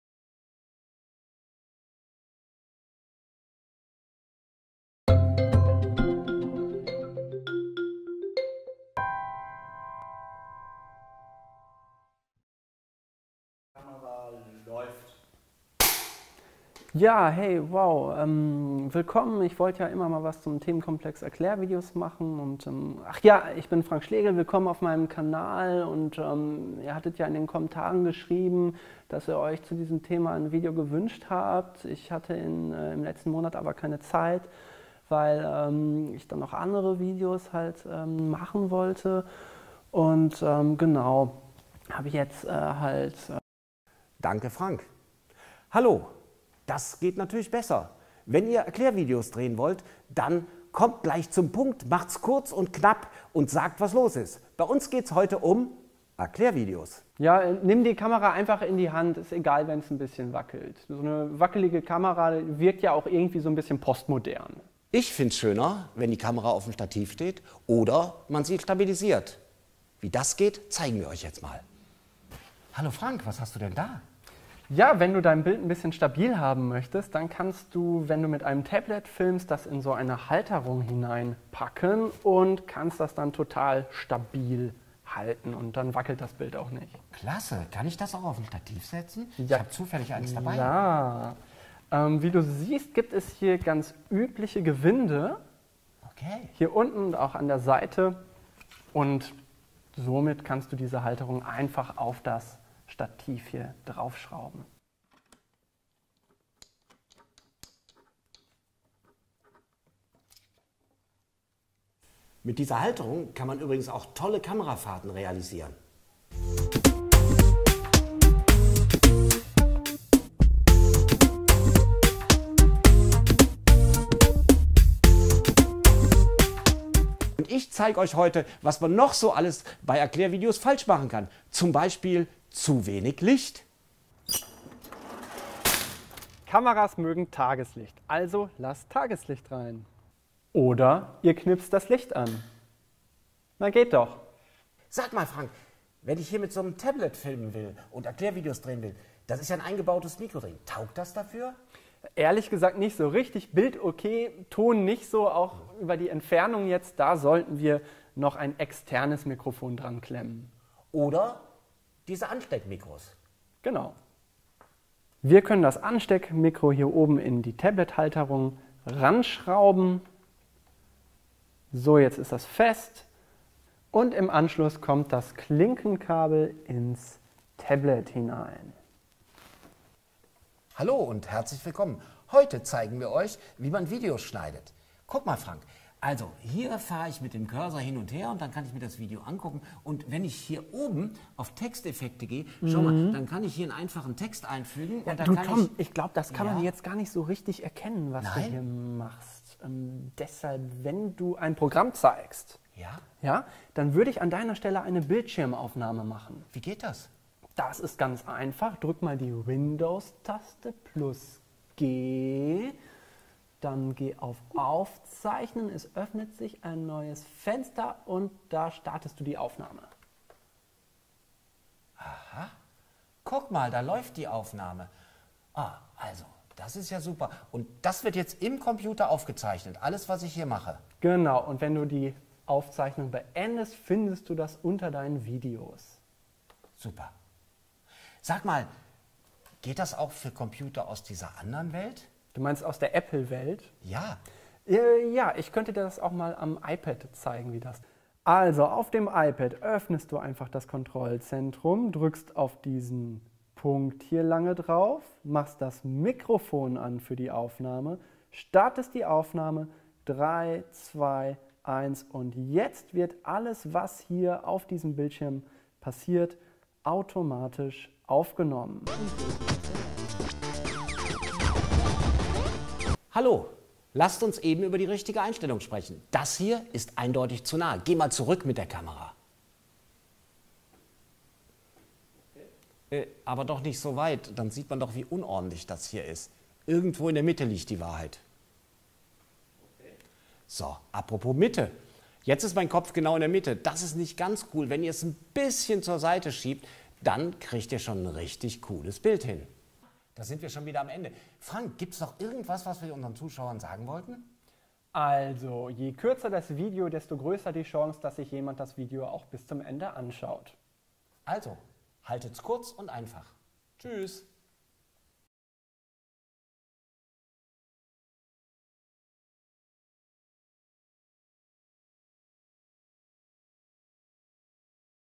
Tutorials: 7 Do`s and Dont`s beim Dreh von Erklärfilmen
Tutorials sind eine gute Möglichkeit, Bibliotheks-Services wie die Onleihe oder die Suche im Bibliothekskatalog zu erklären. Was es beim Dreh von Tutorials zu beachten gilt, zeigen euch die Medienpädagogen Frank Schlegel und Tom Birke in unserem kurzen Video anhand von 7 Do`s and Dont`s. Mehr über die Arbeit der Fachstelle für Öffentliche Bibliotheken NRW auf unserem Blog